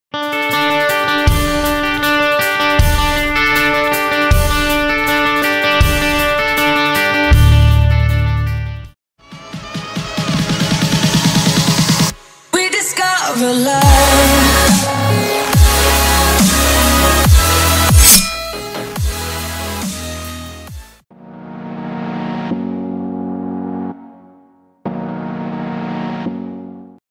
CHANGE. IS. COMING. AND IT'S BIG!
We are officially changing our name to Survival Islanders in honor of our favorite world of Survival Island. Don't worry we will still release VR, Gaming, and Funny videos.